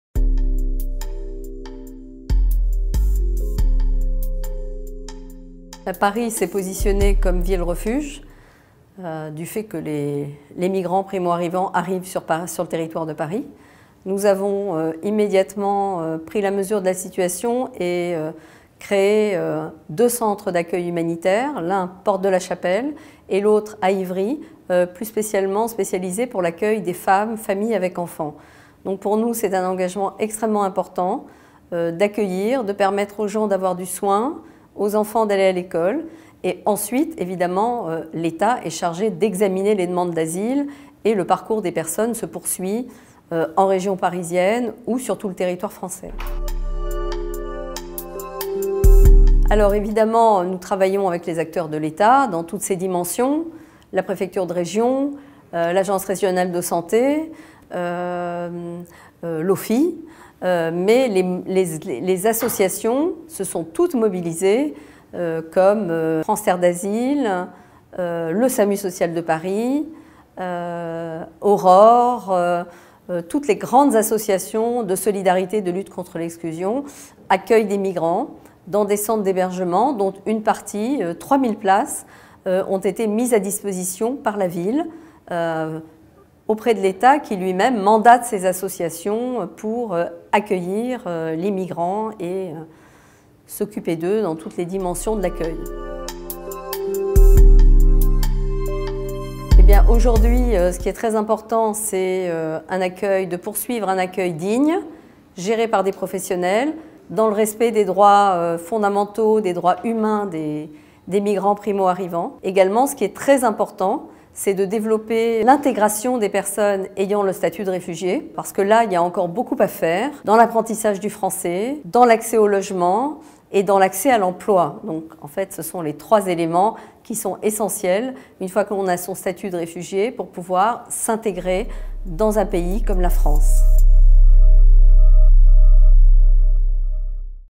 Questions à Dominique VERSINI (Mairie de Paris) - politiques d’accueil des demandeurs d’asile - cese
Questions à Mme Dominique VERSINI, adjointe à la maire de Paris, chargée des solidarités, lutte contre l'exclusion, accueil des réfugiés et protection de l'enfance à la mairie de Paris, auditionnée par la Section des affaires européennes et internationales du CESE dans le cadre de la saisine : "Parcours et politiques d’accueil des demandeur.euse.s d’asile dans l’Union Européenne".